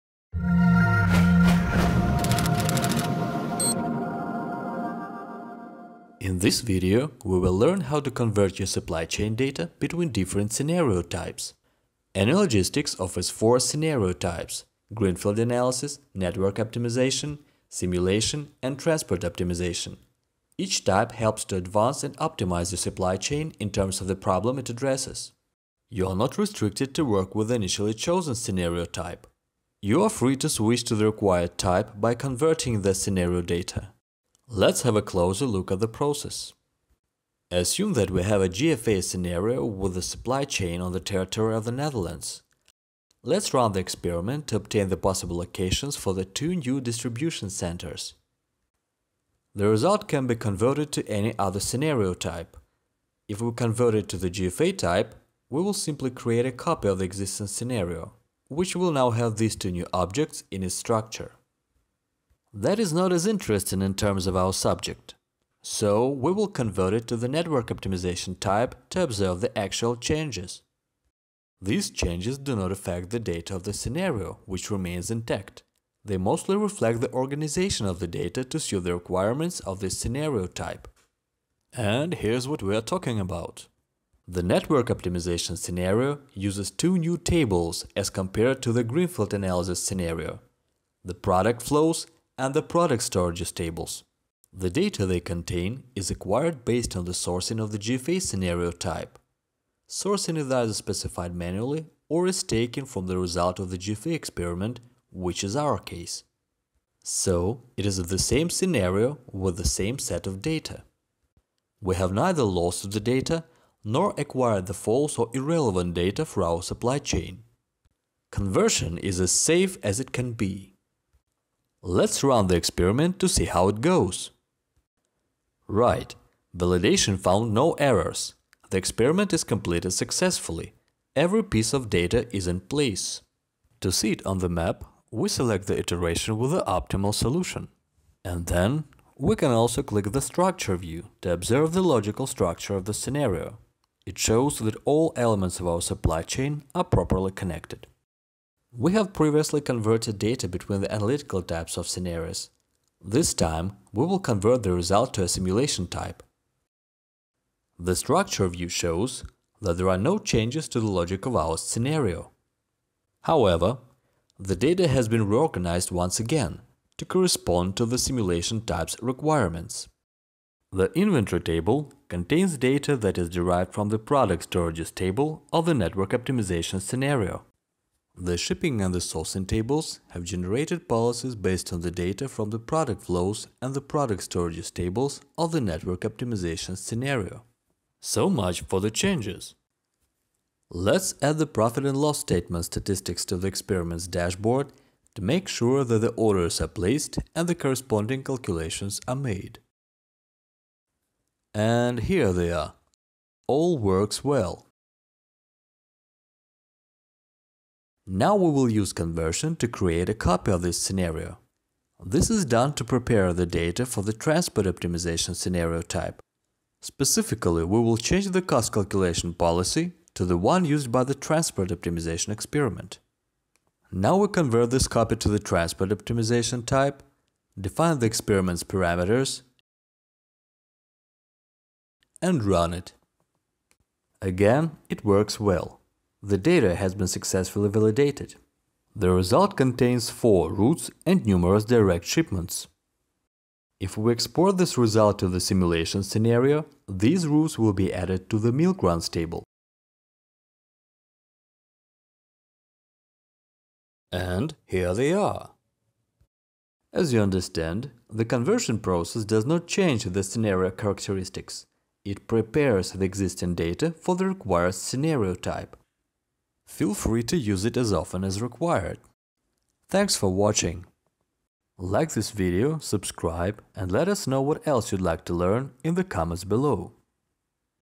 Converting scenario data
This video shows how to convert data between different scenario types.